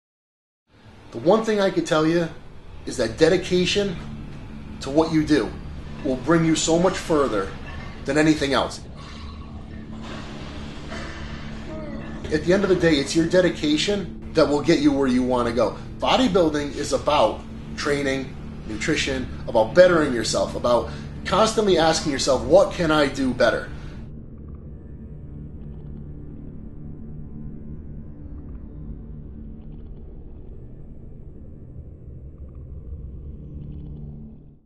Evan "Ox" Centopani Returns to Animal
Animal proudly announces the return of IFBB Pro Evan "Ox" Centopani. Welcome home, Big Ev.